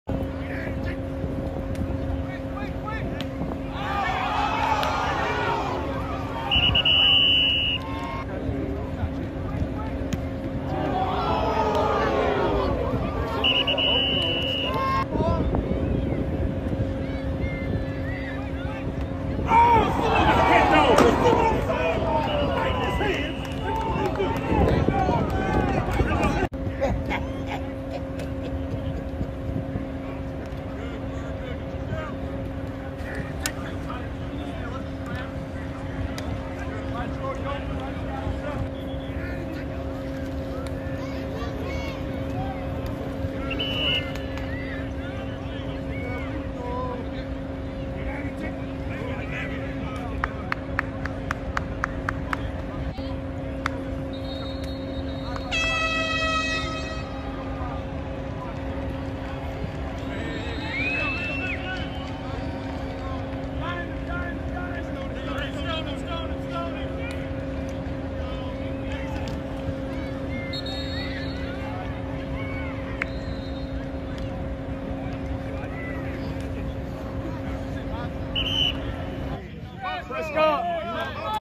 Cowboys QB Dak Prescott 1st INT in camp. Not Diggs & Gilmore, but Rookie CB Eric Scott
Dallas Cowboys QB Dak Prescott throws his first interception after WR Simi Fehoko failed to hold on to the catch. Not Trevon Diggs and not Stephon Gilmore with the interception but Rookie CB Eric Scott Rookie with the first interception of Dallas Cowboys training camp session one.